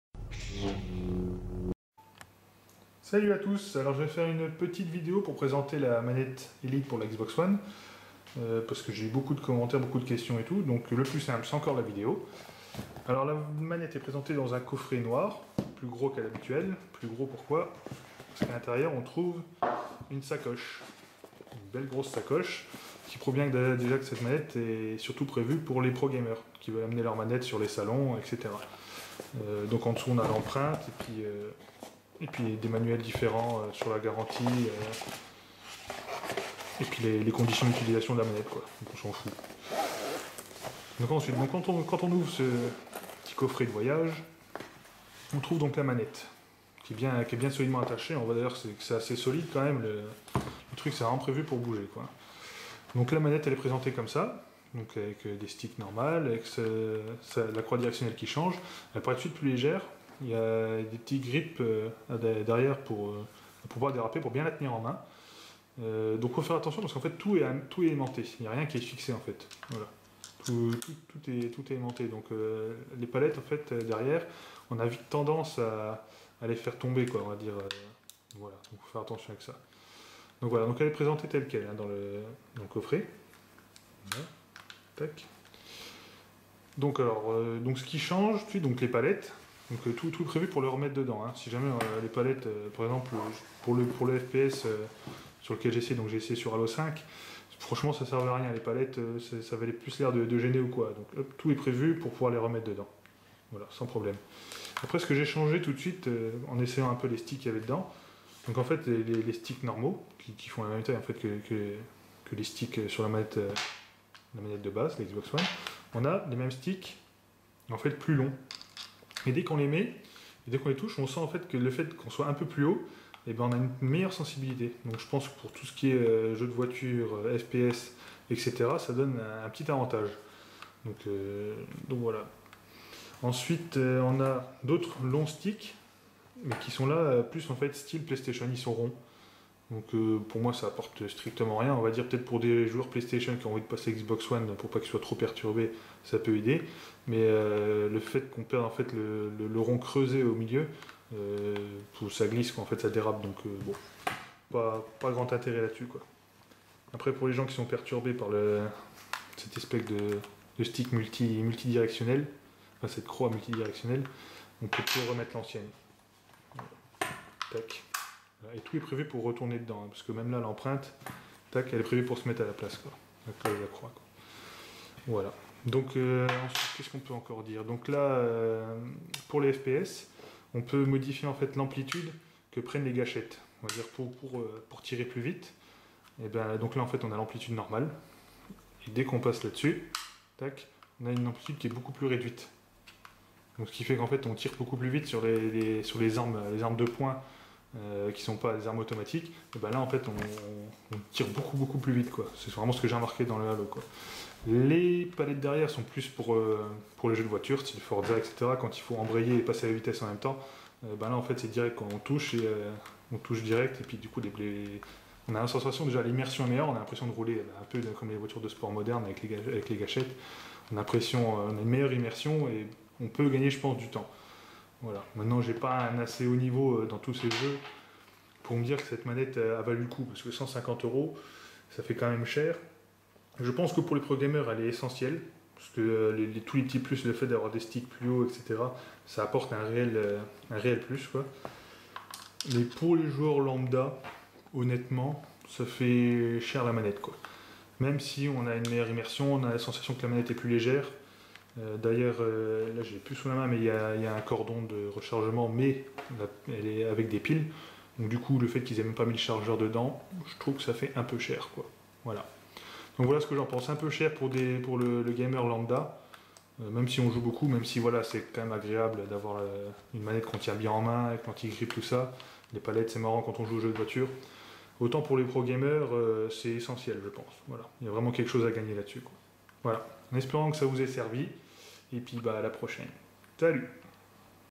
Manette Elite XBox One - Le test de BAM Valmont
BAM Valmont teste pour nous la nouvelle manette Elite de la Xbox One